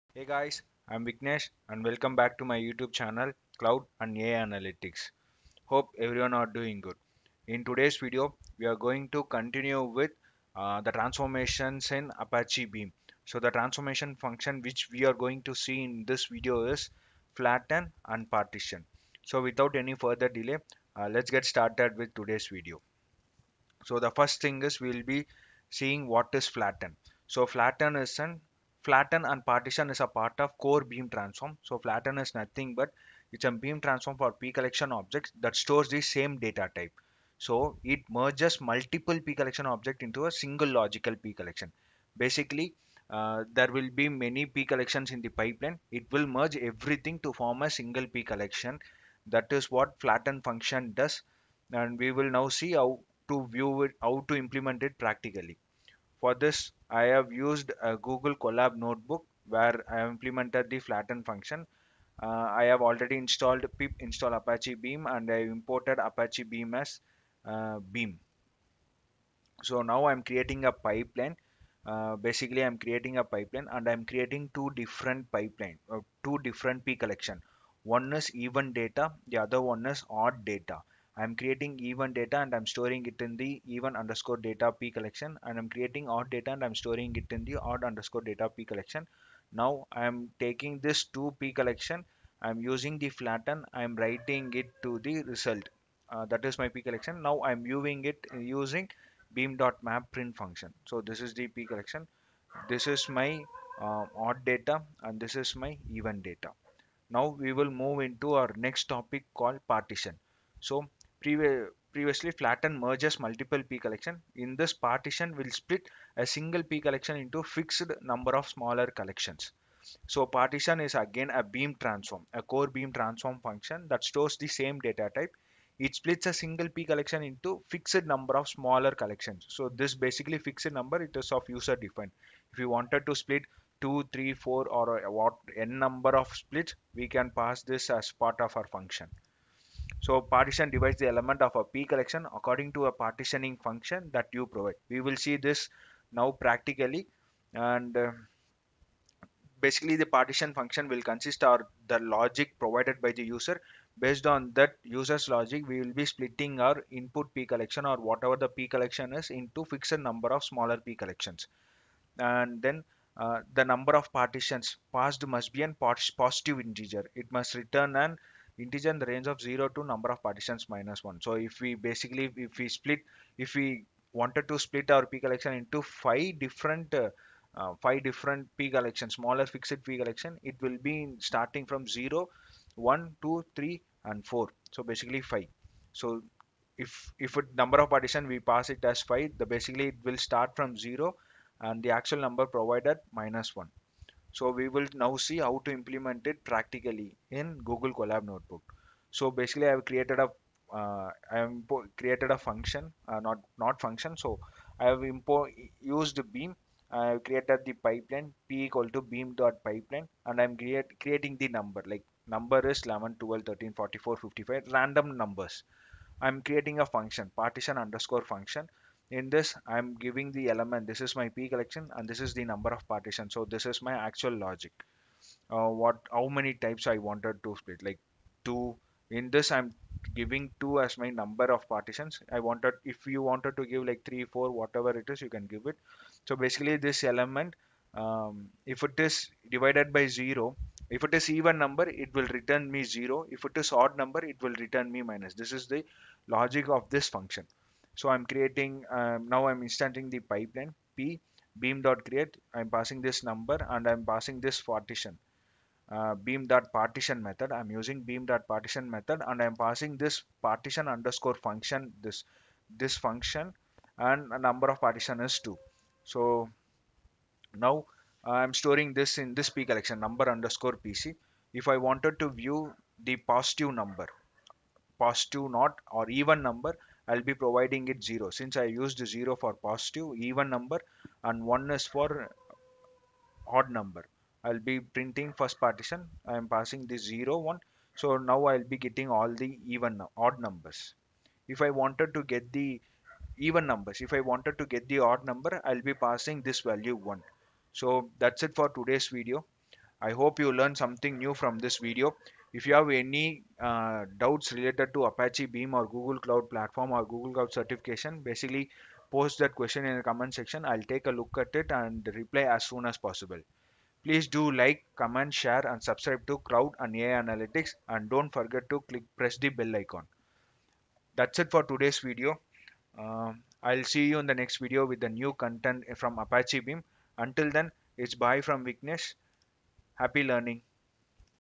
Tutorial 3d – Flatten and Partition Transform in Apache Beam
In this videos we are going to discuss about what is Transformations - Flatten and Partition in Apache Beam.

Flatten:
● Flatten is a Beam transform for PCollection objects that store the same data type.
● Merges multiple PCollection objects into a single logical PCollection.

Partition:
● Partition is a Beam transform for PCollection objects that store the same data type. It splits a single PCollection into a fixed number of smaller collections.
● Partition divides the elements of a PCollection according to a partitioning function that you provide.

1. LIKE
2. SHARE